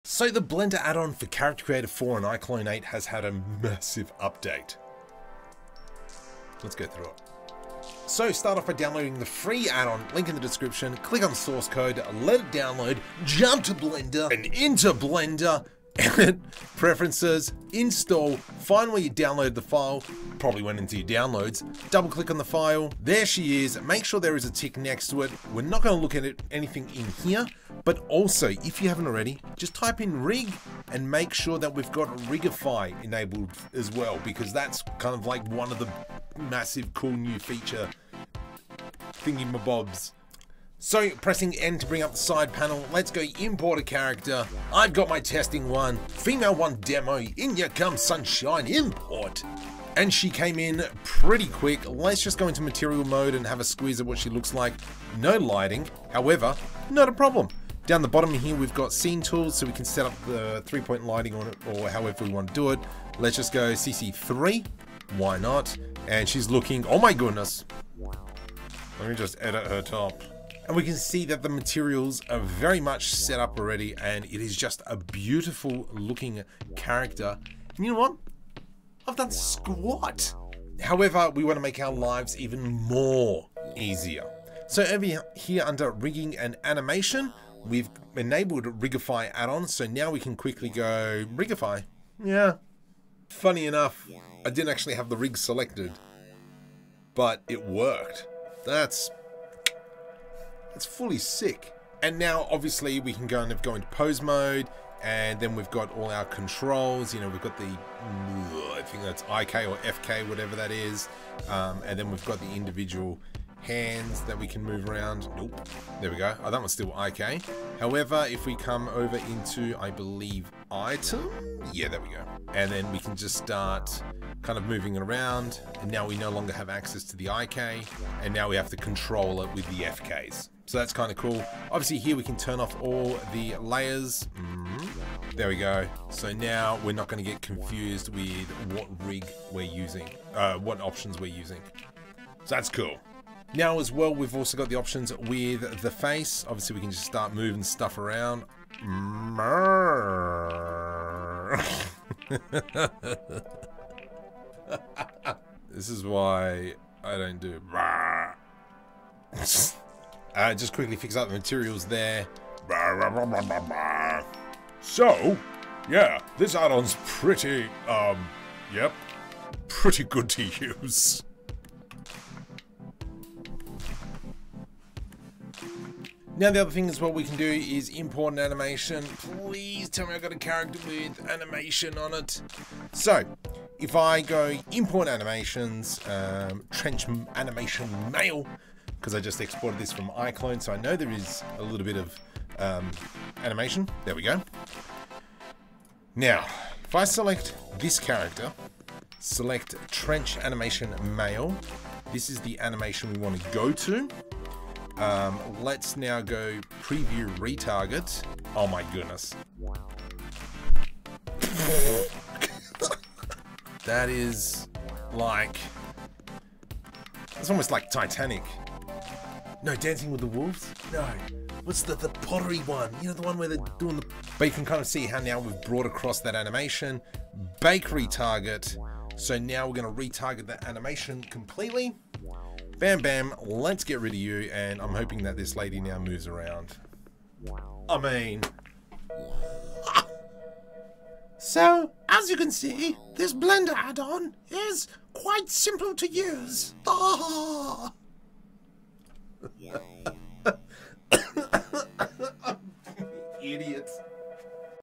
Free Accurig/Character Creator 4 to Blender Addon
Importing Characters from Acurig or Character Creator 4 or iClone 8 to  Blender has never been so easy.